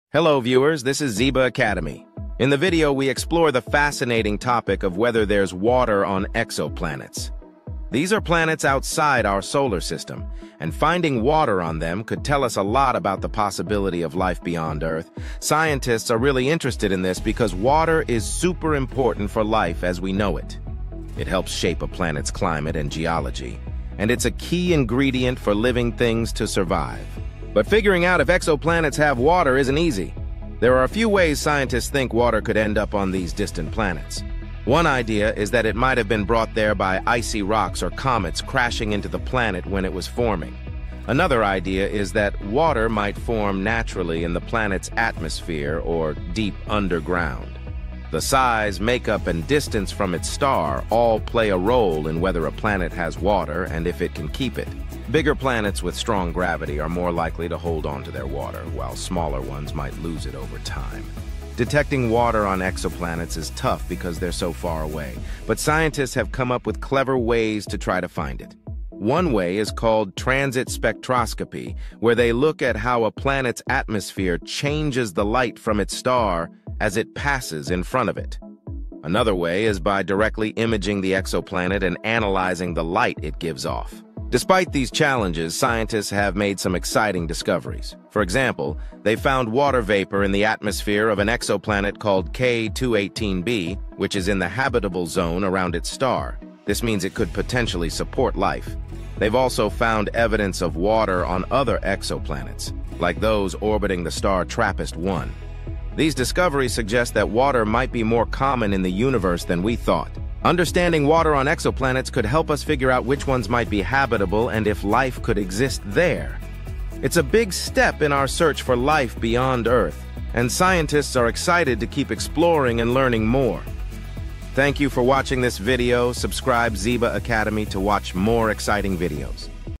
Is There Water on Exoplanets?
In the vast expanse of the universe, exoplanets have captured the imagination of scientists and enthusiasts alike. These celestial bodies, orbiting stars beyond our solar system, hold the potential to unravel some of the greatest mysteries of our existence. One of the most intriguing questions surrounding exoplanets is whether they harbor water, a vital ingredient for life as we know it.

Understanding the presence of water on exoplanets is crucial in the search for habitable worlds beyond our own. Water not only serves as a prerequisite for life but also plays a significant role in shaping the planet’s climate and geology. In this video, we delve into the theoretical possibilities of water formation on exoplanets, explore the mechanisms for its delivery and retention, and examine the observational techniques employed by scientists to detect water on these distant worlds.